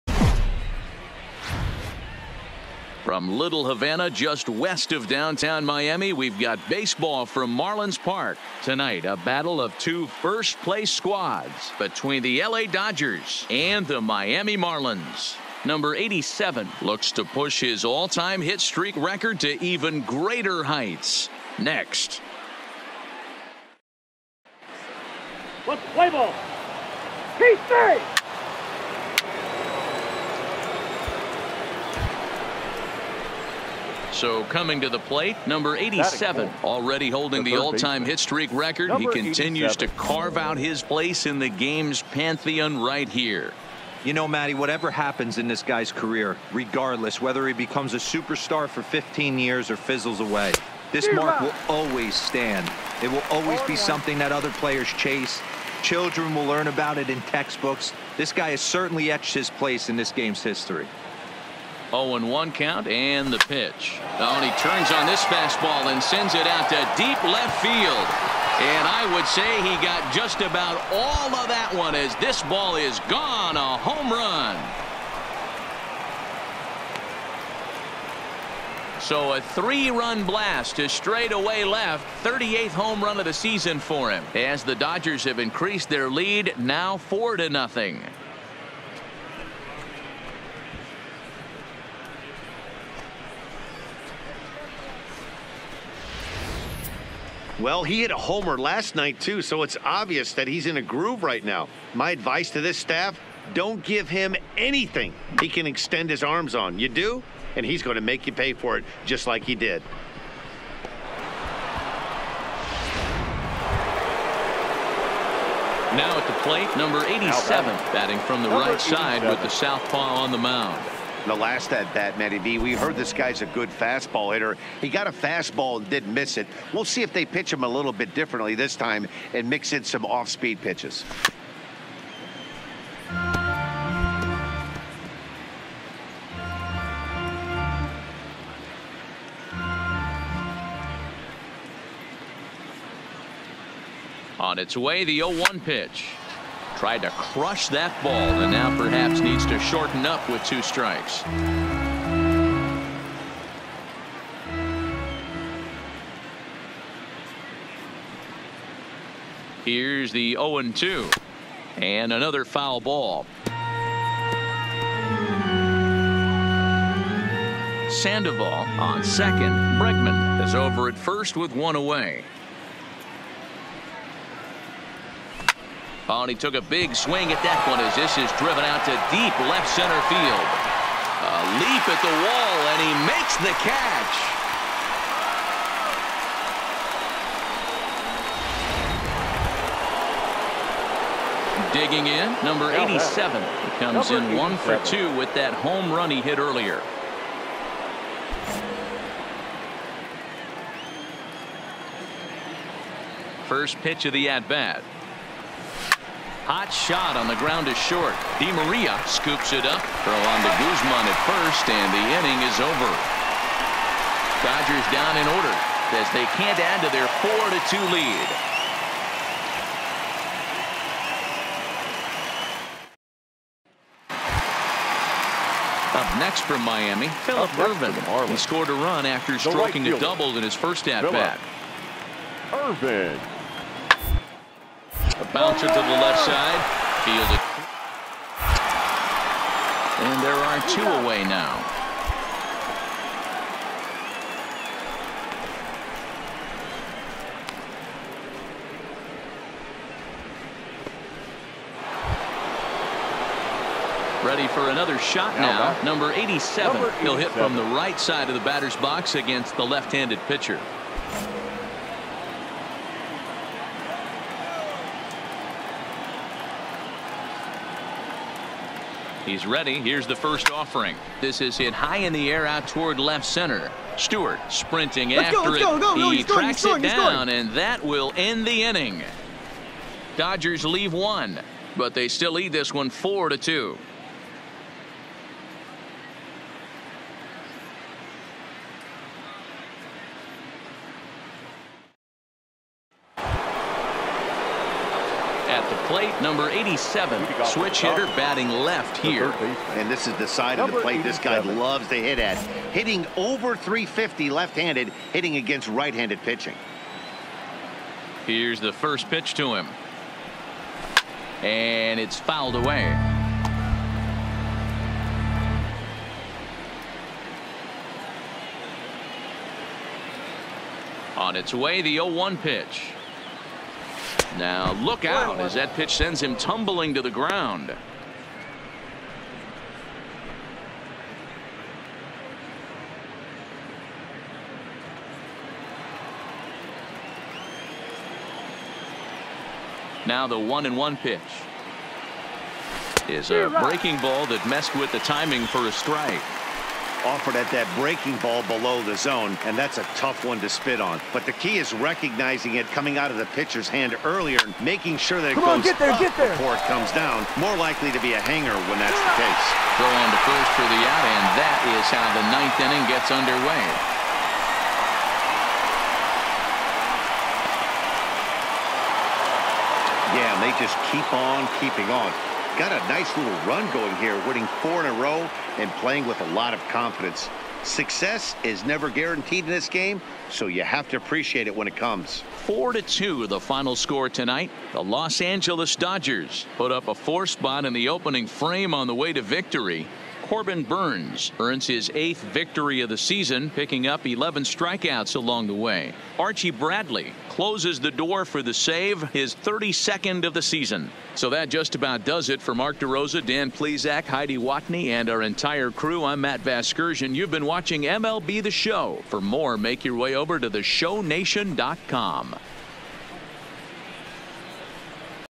Series Win
The Dodgers go for the series win against the Marlins